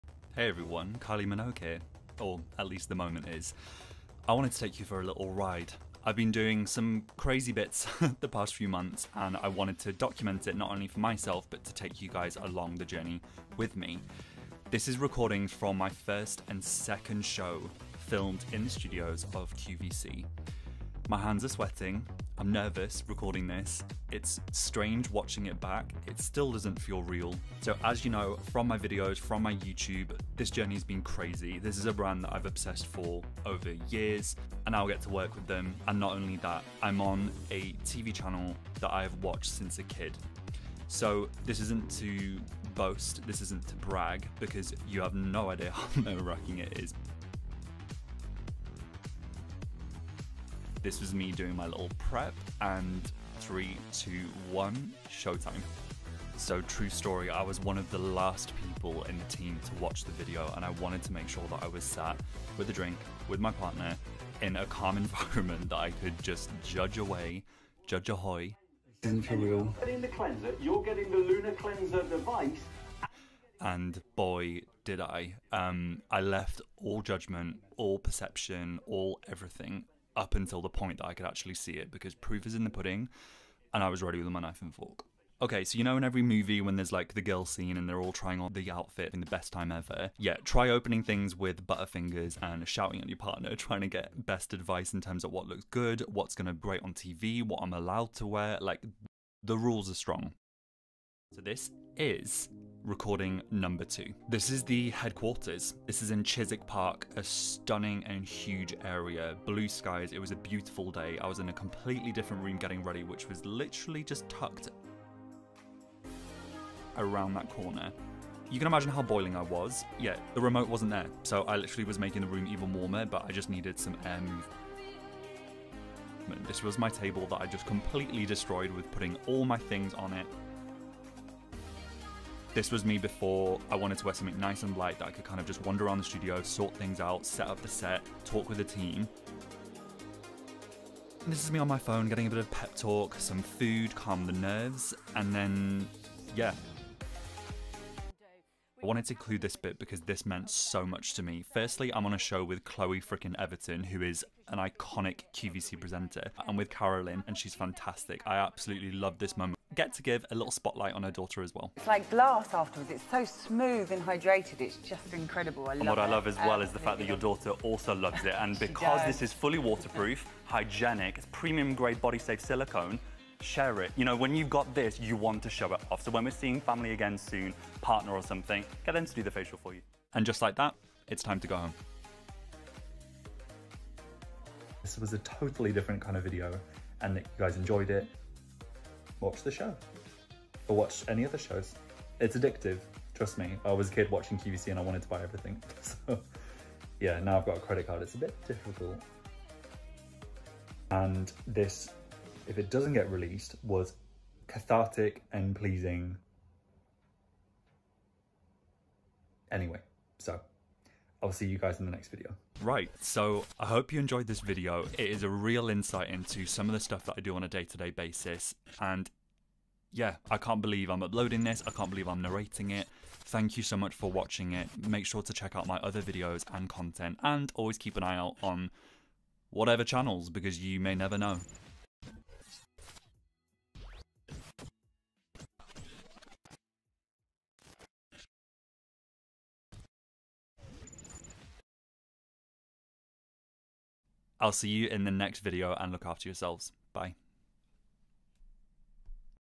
QVC behind the scenes - first time on tv vlog
QVC behind the scenes , sounds cool? Sit down and grab a cup of tea/anything and join my journey through two of my first studio filmed shows for TV with some amazing talents like Simon Biagi, Chloe Everton and Carrelyn Gardner.

I hope you enjoy this format of video, I wanted to film what I could and document this crazy chapter from work and let me know what you'd like to see in the future!

I touch on this in the video, but this is by no means to ever brag or gloat during thes tough times. You only see what I choose to show, the sleepless nights, stress, anxiety and pulling strength from places you didn't know existed to be able to manage some of these projects are things that are hard to imagine or empathise if it isn't you amongst it all. So if you are watching this and you are either going on QVC, have done QVC, or are always on QVC.. I admire you, sending a huge and good luck on your next show!